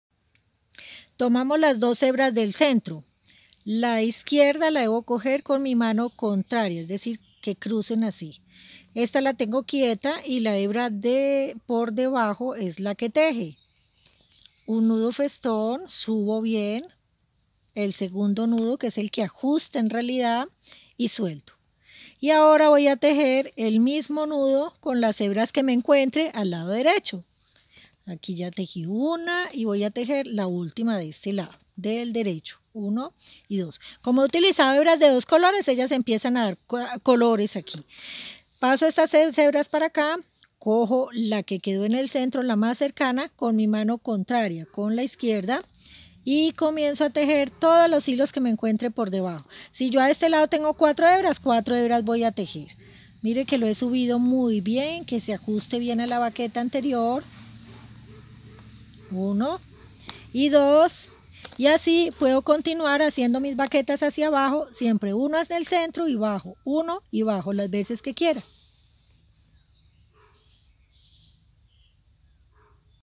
Macramé BAQUETA 2da hilera
Baqueta de ADENTRO hacia AFUERA es decir se comienza por el CENTRO,  primero hacemos la baqueta que se dirige al lado Derecho y luego la baqueta que se dirige hacia el lado Izquierdo